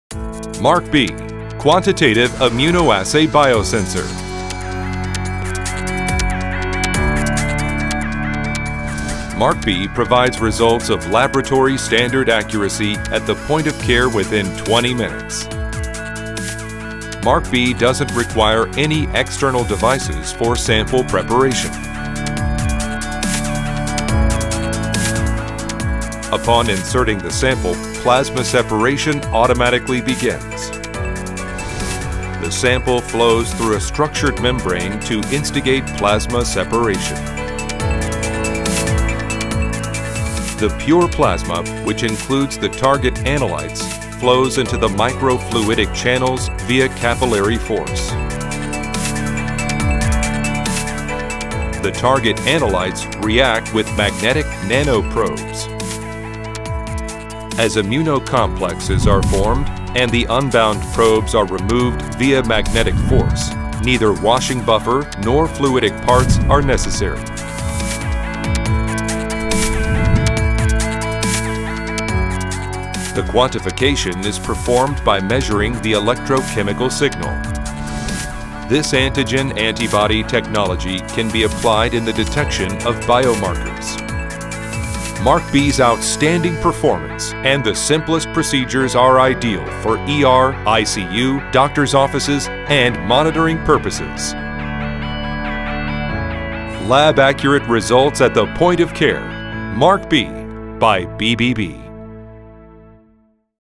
MARK-B™ ElectroChemical ImmunoAssay from BBB_eng narration
[English narration version.] With ‘MARK-B™’, all you need to measure the concentration of biomarkers in the blood are a drop of blood from a finger-prick and 20 minutes. After inserting a test cartridge and applying your blood sample, BBB's patented plasma separation technology enables the complete removal of blood cells from a drop of finger-prick blood without any external field. Once the plasma is extracted, the amount of biomarker proteins in the plasma is measured by BBB's new technique, "MESIA; Magnetic force-assisted Electrochemical Sandwich ImmunoAssays". Additional handling, such as applying buffer solution for washing, is not required. The whole process is operated in a fully automated manner on the cartridge. By using this patented technique, significant biomarkers in the plasma are detectable with high sensitivity.
BBB begin with cancer markers; PSA, AFP, CEA, CA125, CA19-9 starting from 2020. Also, other products such as CVD, sepsis, hormones, and infectious disease test markers will follows soon.